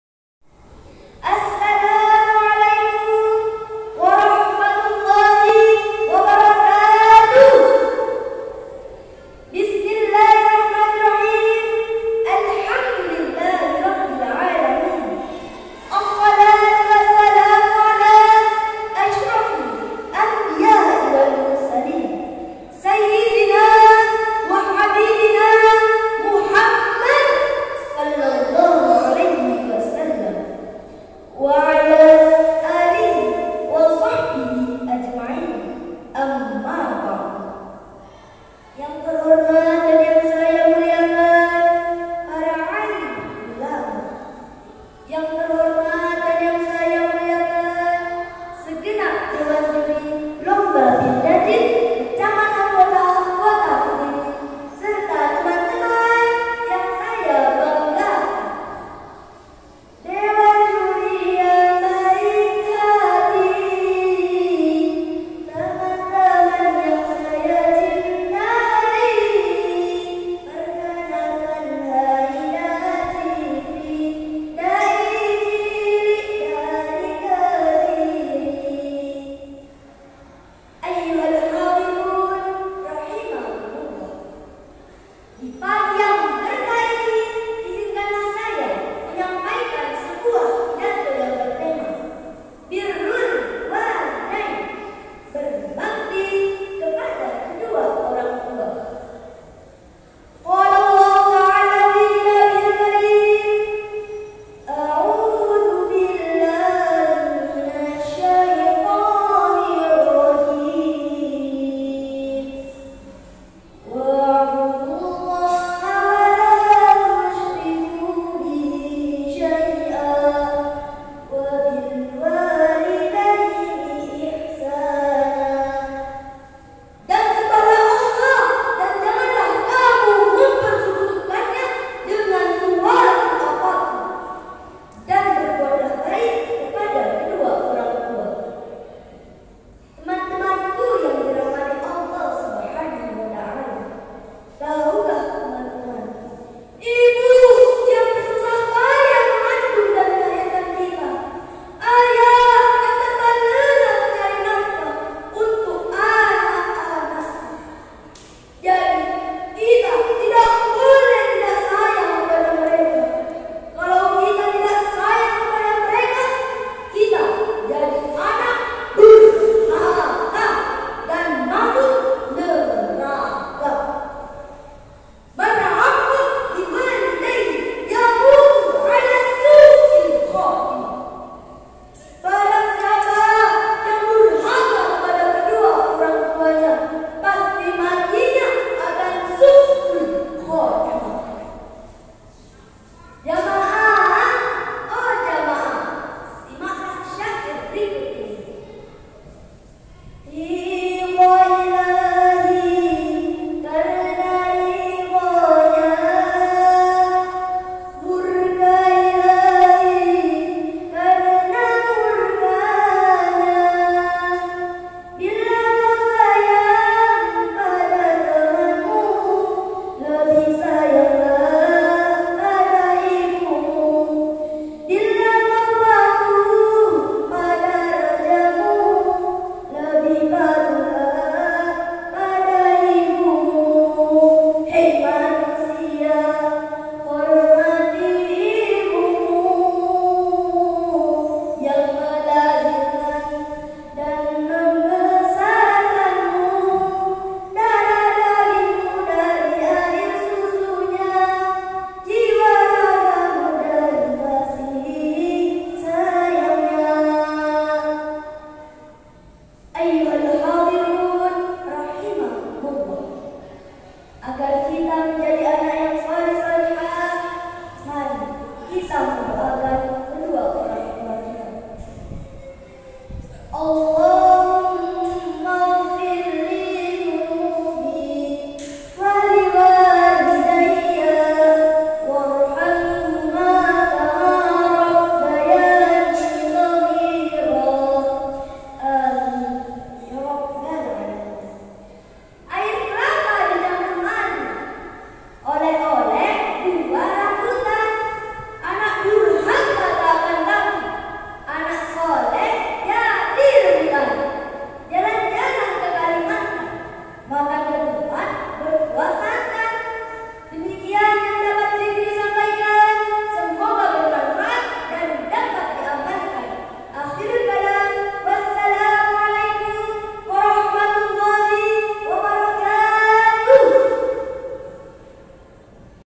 Program Keagamaan Ekstrakurikuler Pildacil
Kegiatan Ekstrakurikuler Pildacil di SD Islam Al Huda 2 dilaksanakan setiap hari jum'at pukul 14.00 WIB sampai dengan 15.00 WIB.

Tujuan diadakannya kegiatan ekstrakurikuler pildacil ini adalah untuk mencetak generasi muda yang cakap, terampil dan percaya diri dalam berdakwah li i'lai kalimatillaah serta dapat berdakwah dengan akhlakul karimah yang rahmatan lil 'alamin.

Dalam proses pembelajarannya, murid tidak hanya di ajari tentang bagaimana dapat berdakwah dengan baik, namun juga di ajarkan untuk dapat memiliki kemampuan dan keterampilab public speaking yang baik dan bagus.

Dari talent ini, murid-murid SD Islam Al Huda 2 telah menorehkan berbagai prestasi di tingkat kota, karisidenan dan provinsi.